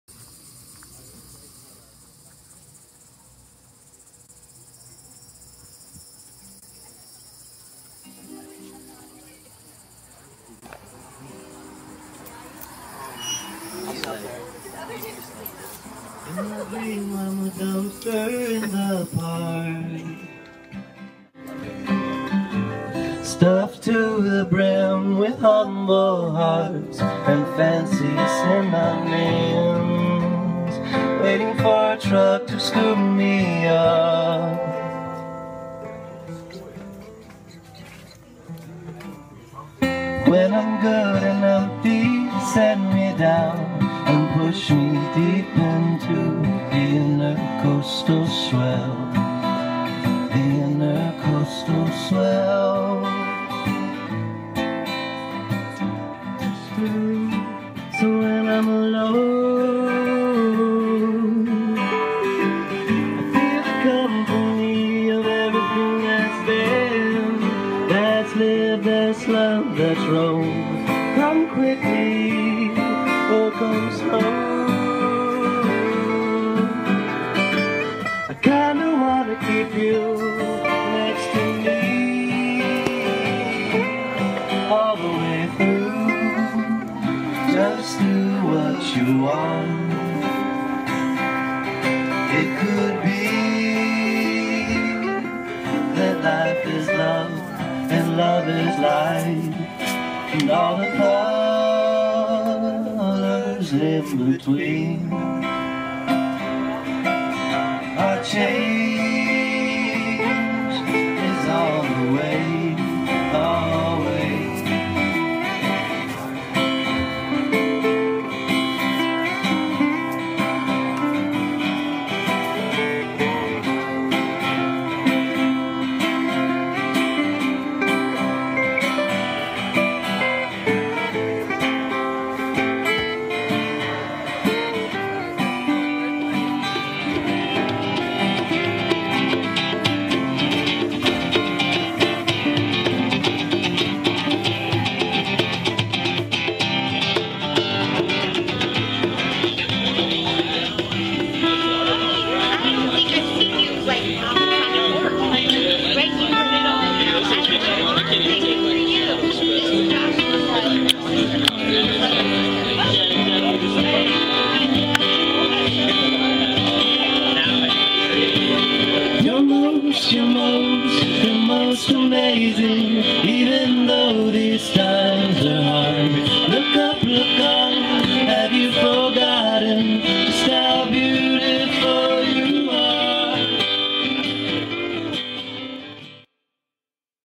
Intercoastal Swell live @ Coffee Bar Blue Door Stuart, Fl.
From Joshua Kumrits's debut performance with Intercoastal Swell and Alex Baranov at Coffee Bar Blue Door. A special evening with friends under courtyard lights with original songs from the heart. This was a sweet first show! Enjoy the video :)